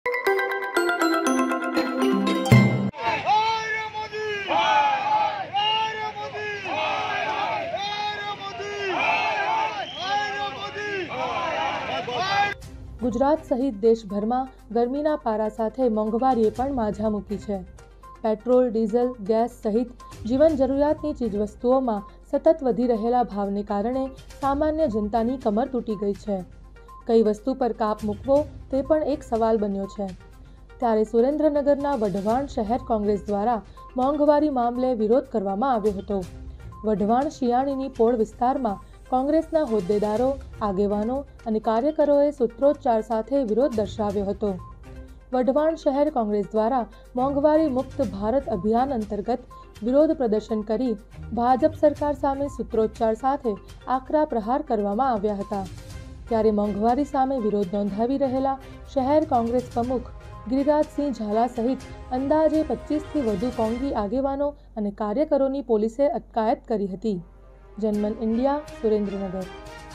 વઢવાણ શહેર કોંગ્રેસ દ્વારા મોંઘવારી મુક્ત ભારત અભિયાન અંતર્ગત વિરોધ પ્રદર્શન, કાર્યકરોની અટકાયત
જરાત સહિત દેશભરમાં ગરમીના પારા સાથે મોંઘવારીએ પણ માઝા મુકી છે. પેટ્રોલ-ડીઝલ, ગેસ સહિત જીવન જરૂરિયાતની ચીજ વસ્તુઓમાં સતત વધી રહેલા ભાવને કારણે સામાન્ય જનતાની કમર તુટી ગઈ છે. કઈ વસ્તુ પર કાપ મુકવો તે પણ એક સવાલ બન્યો છે.